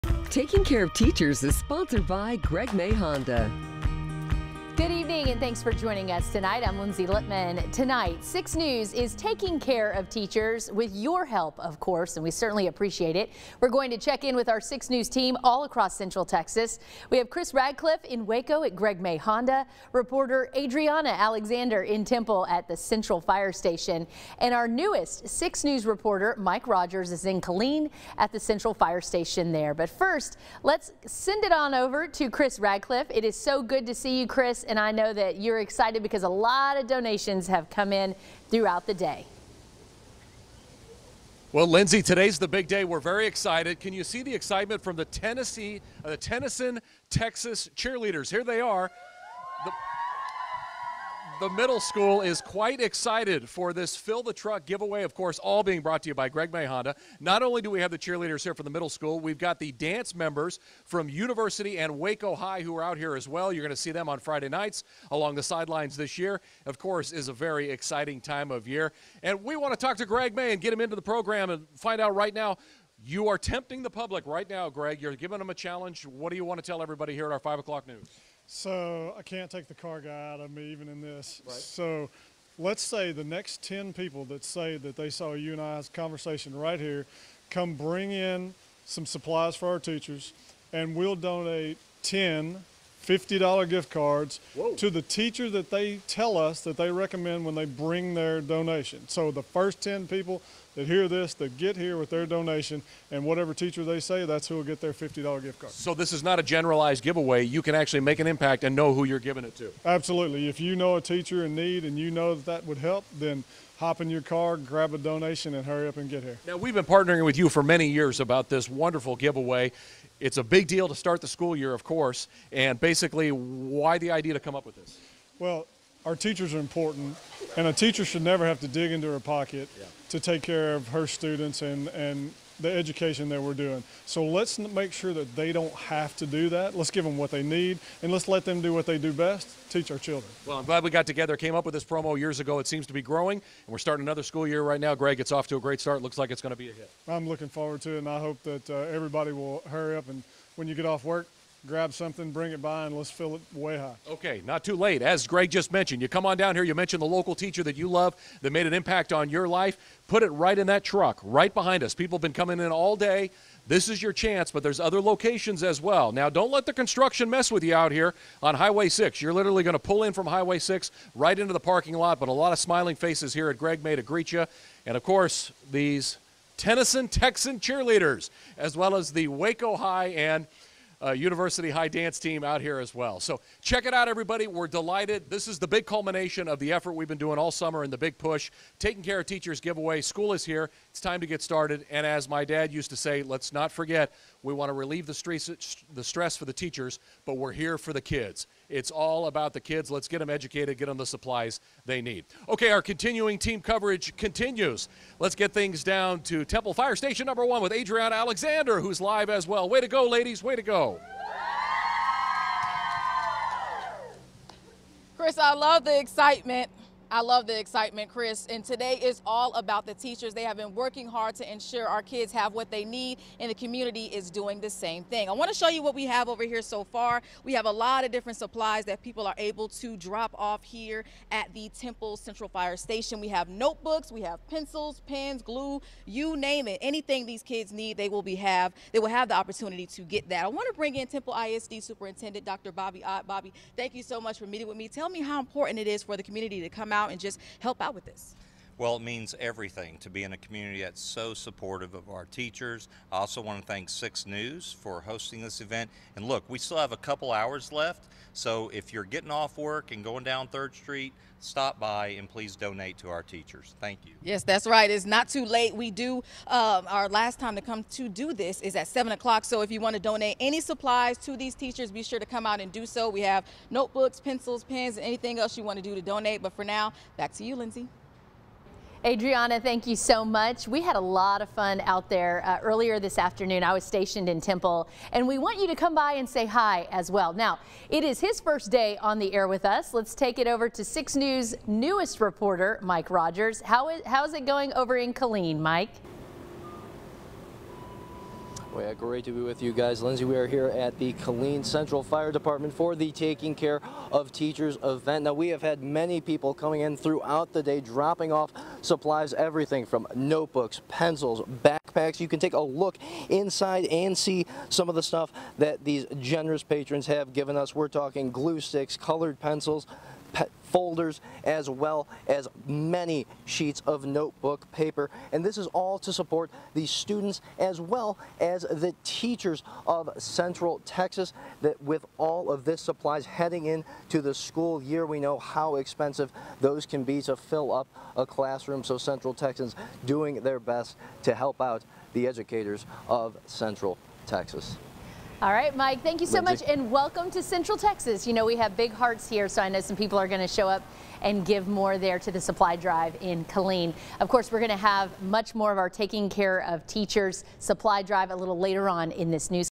Taking Care of Teachers Supply Drive
Donate school supplies in Waco, Killeen and Temple for taking care of teachers